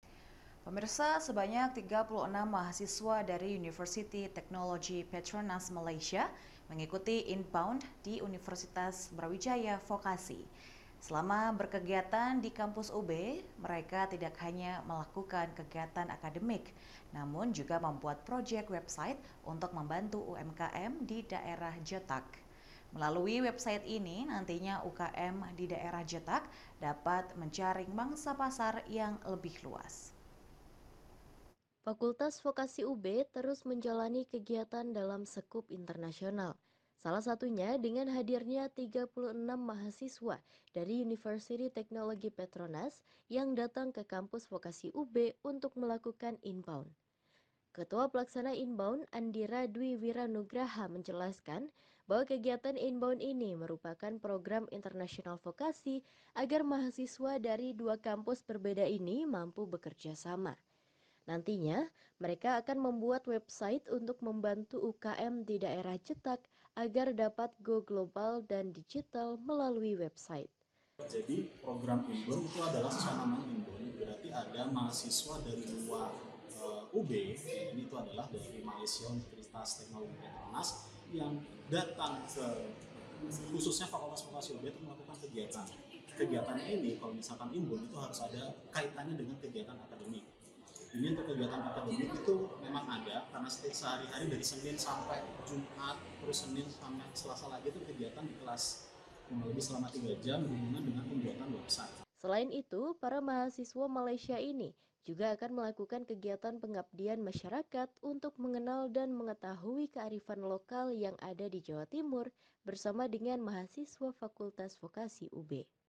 36 MAHASISWA UTP MALAYSIA IKUTI INBOUND DI FAKULTAS VOKASI UB - NEWS POINT
UNIVERSITAS BRAWIJAYA TELEVISI (UBTV)
43 UHF Channel Malang Raya
tv.ub.ac.id